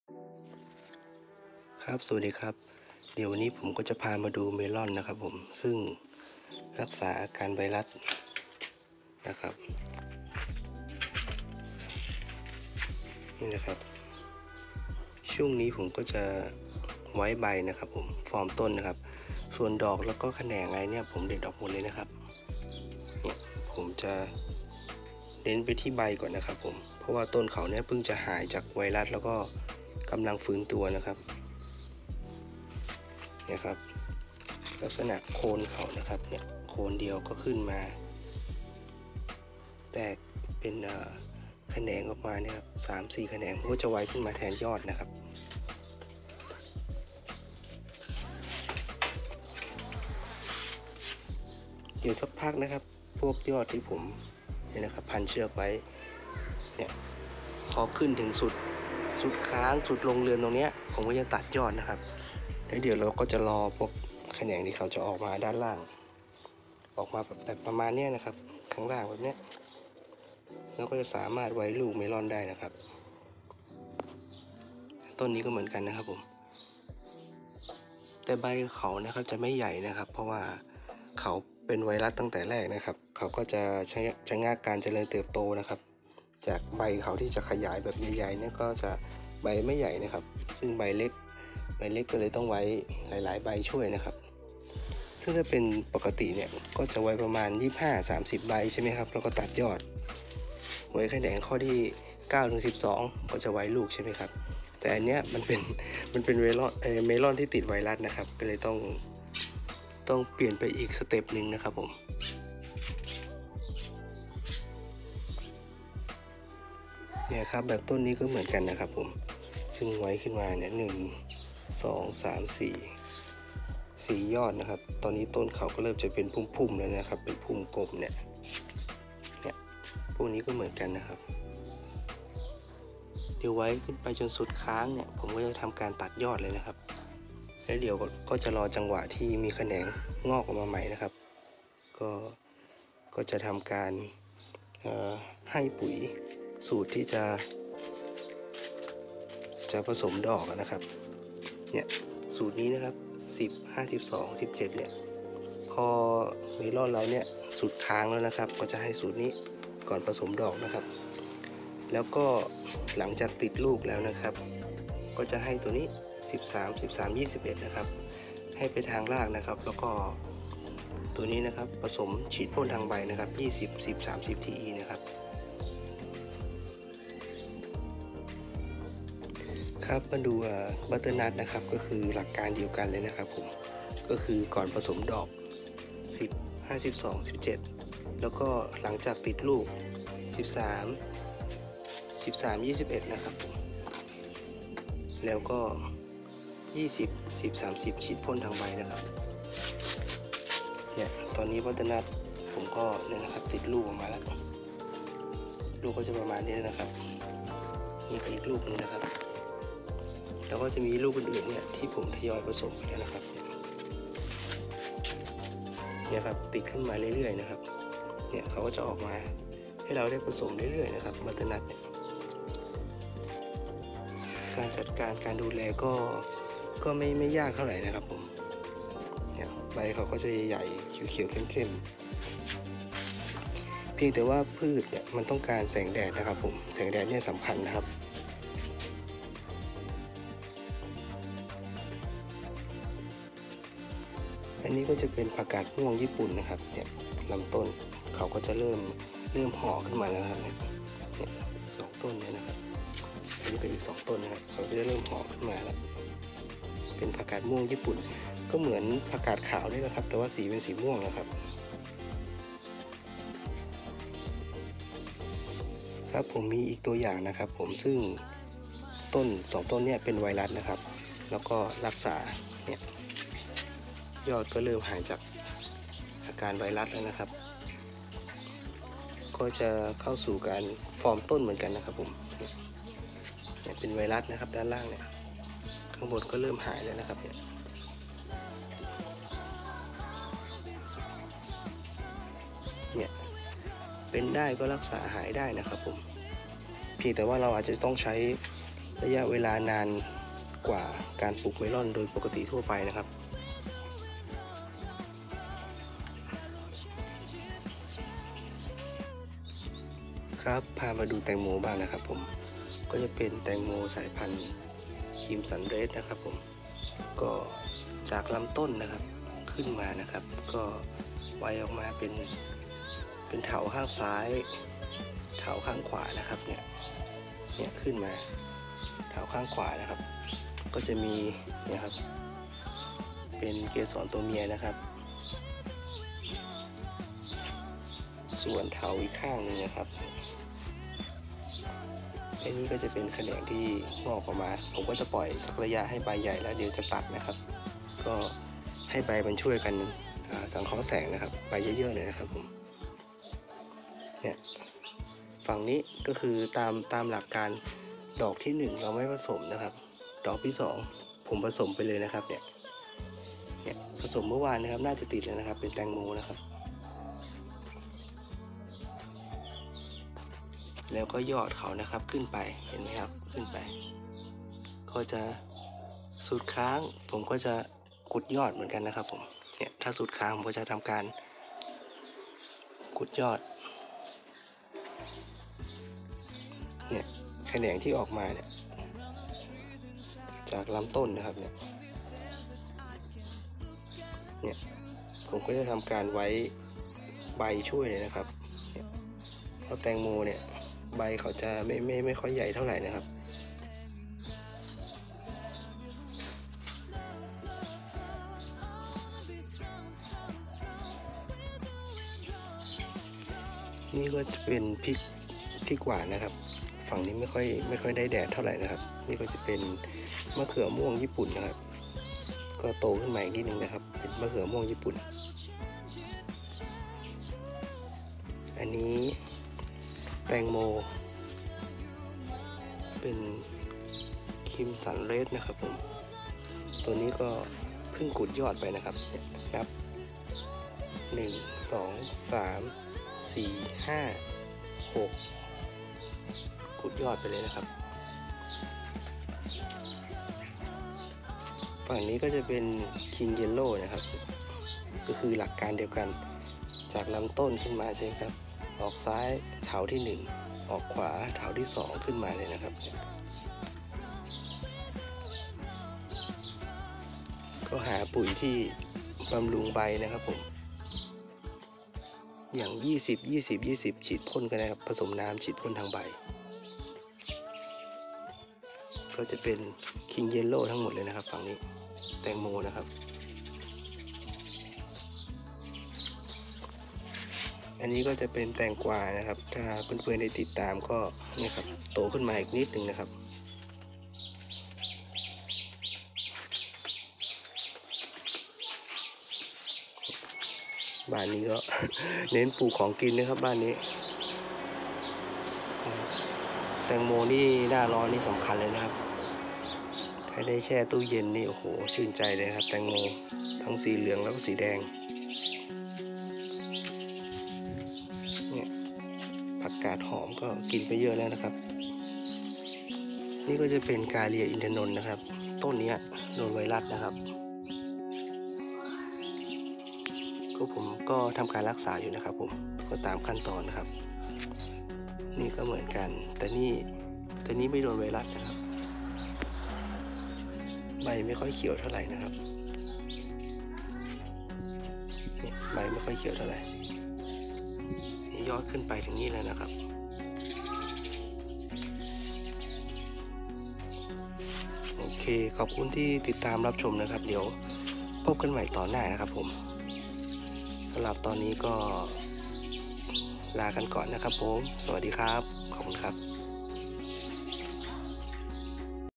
รักษาไวรัสเมล่อนตอนที่6
ตอนนี้เมล่อนของเราอยู่ในช่วงฟอร์มต้นก็เริ่มฟื้นแล้วนะครับแนวทางต่อไปก็คือไว้ใบจนสุดยอดแล้วก็ตัดตัดยอดแล้วก็ให้ปุ๋ยบำรุงดอก และรอผสมดอก ไว้ผลเมล่อนต่อไป...ขอบคุณที่ติดตามรับชมครับ